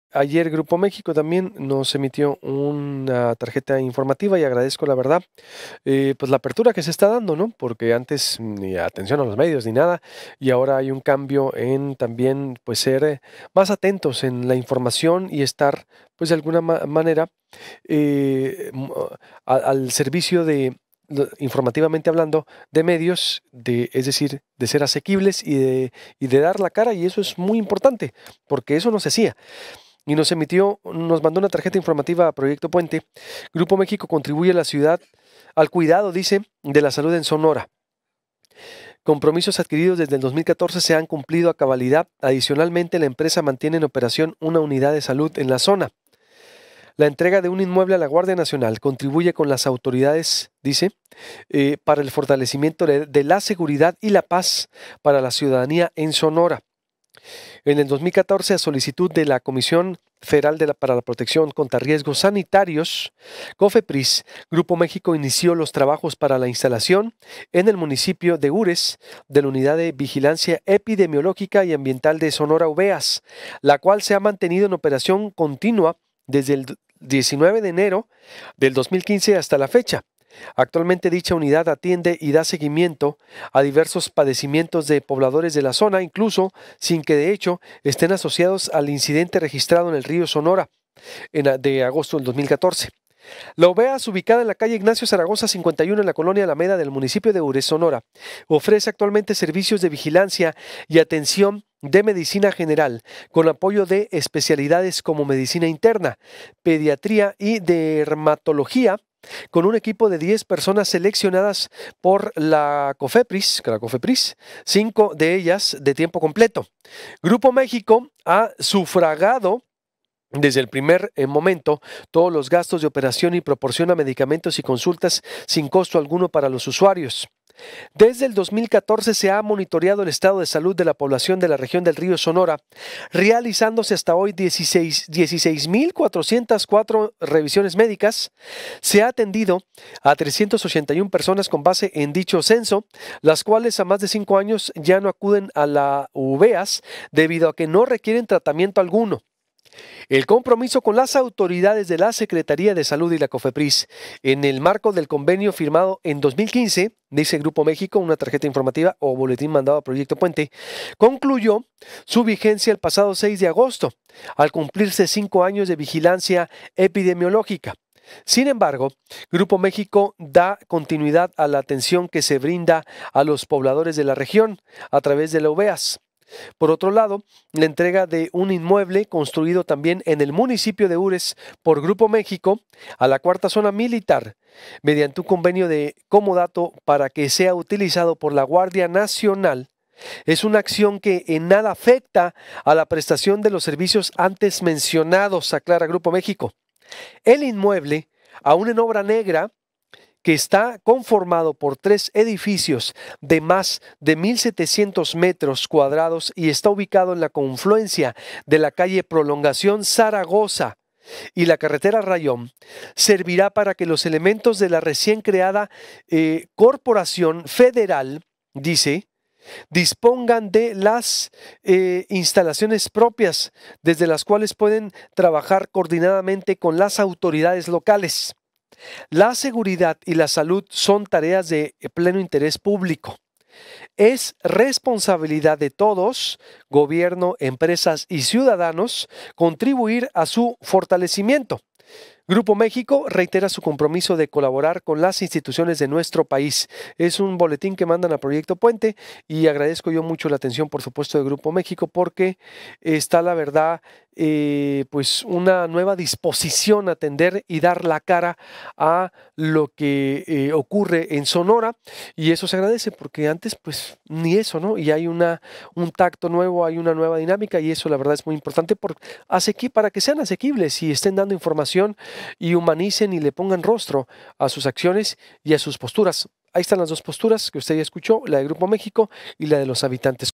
No se ha dejado de atender la salud de habitantes del Río Sonora y continuará: Grupo México
A solicitud de la Comisión Federal para la Protección contra Riesgos Sanitarios (Cofepris), Grupo México en el 2014 inició trabajos para la instalación, en el municipio de Ures, de la Unidad de Vigilancia Epidemiológica y Ambiental de Sonora (Uveas), la cual se ha mantenido en operación desde el 19 de enero de 2015, hasta la fecha.

Actualmente dicha unidad atiende y da seguimiento a diversos padecimientos de pobladores de la zona, incluso sin que, de hecho, estén asociados al incidente registrado en el Río Sonora en agosto de 2014.

La Uveas, ubicada en municipio de Ures, ofrece actualmente servicios de vigilancia y atención de medicina general, con apoyo de especialidades como medicina interna, pediatría y dermatología.